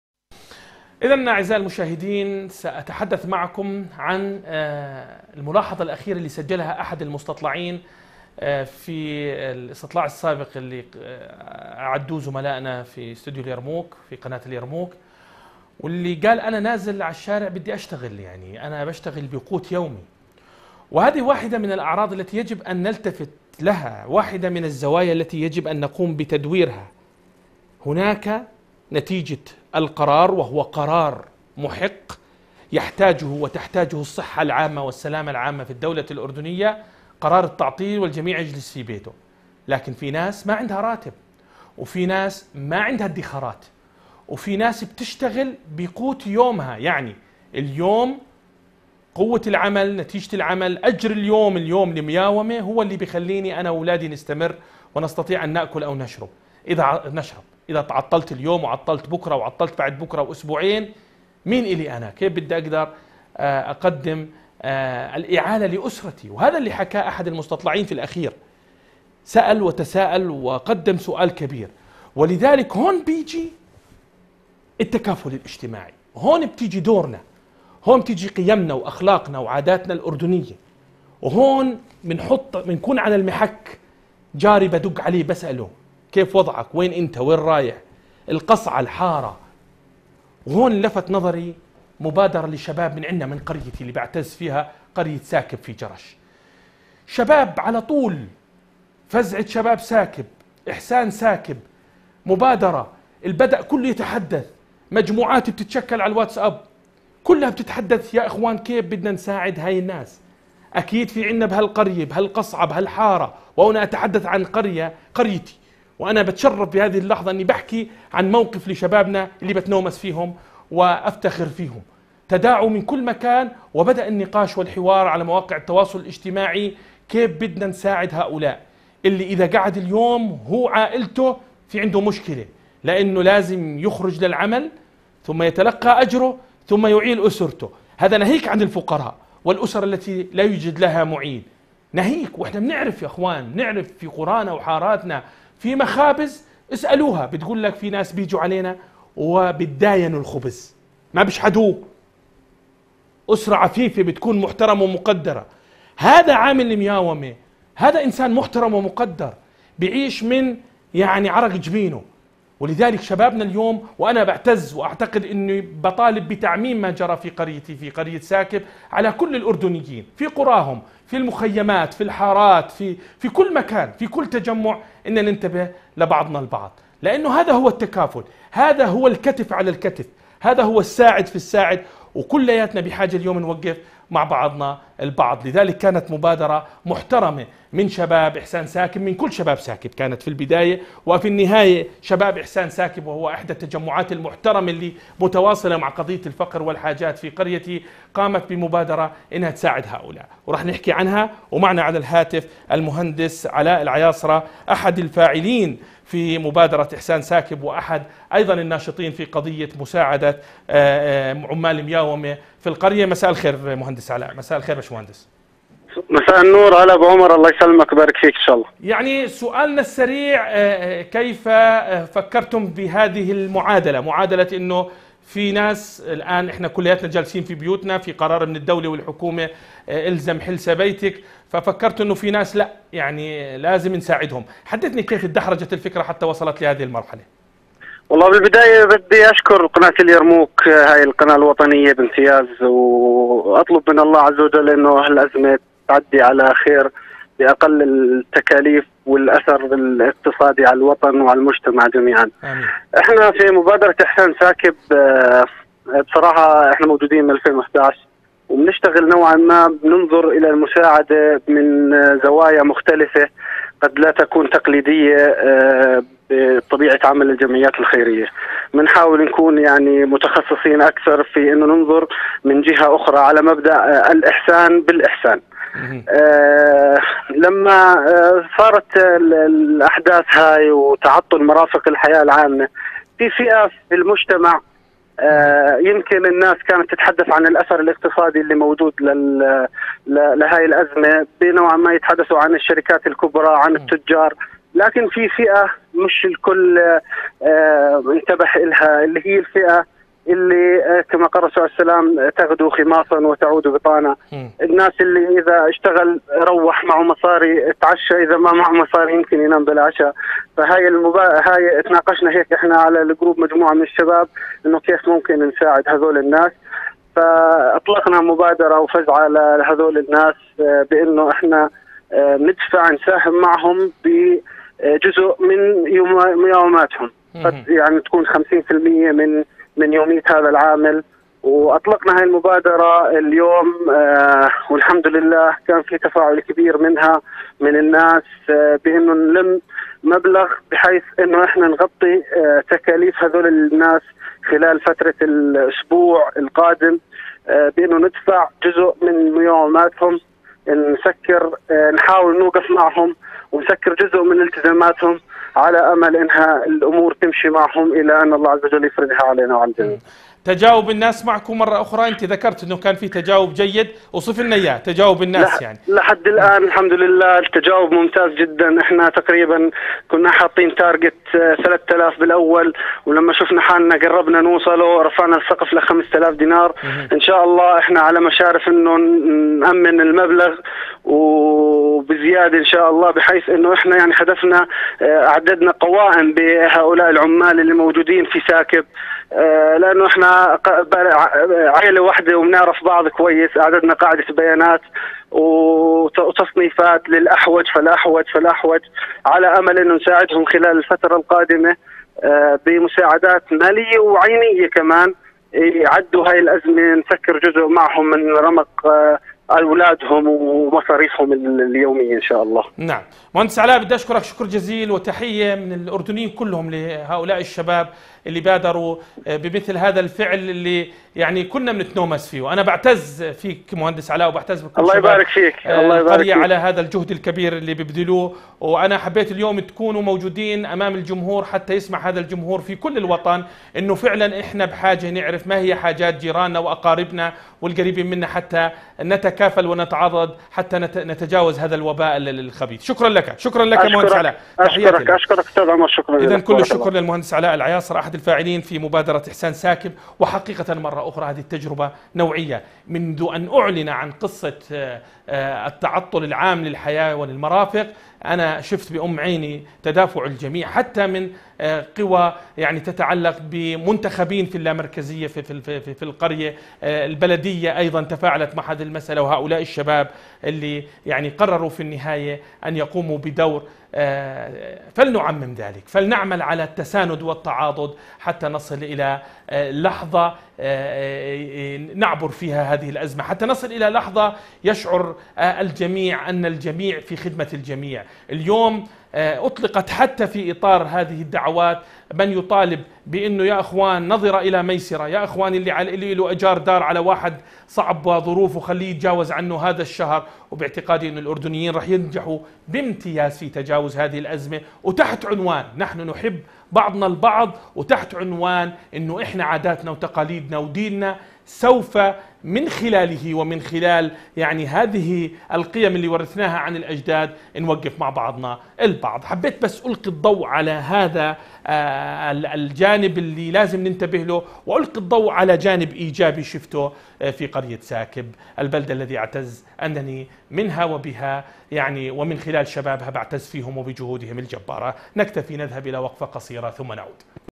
شباب قرية ساكب ينظمون مبادرة للوقوف مع المتضررين من ظروف الحالية / استوديو اليرموك
تقديم : عمر عياصرة